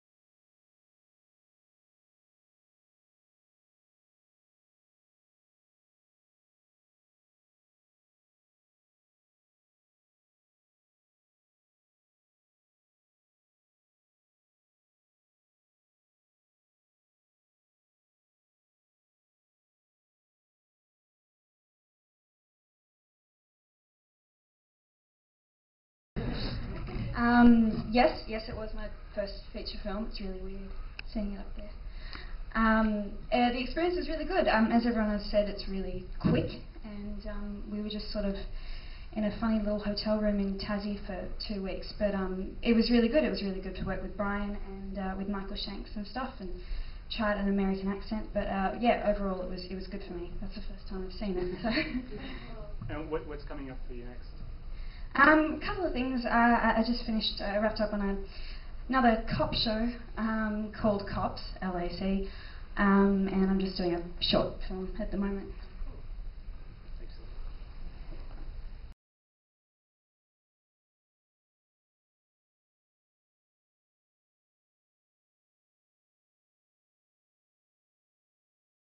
Indiana Evans Arctic Blast 2010
Arctic Blast has its World Premiere at the 5th Canadian Film Festival @ Dendy Quays. Indiana at the Q& A afterwards.
NOTE: 20 plus seconds of no sound at the start & finish due to copywrite & music issues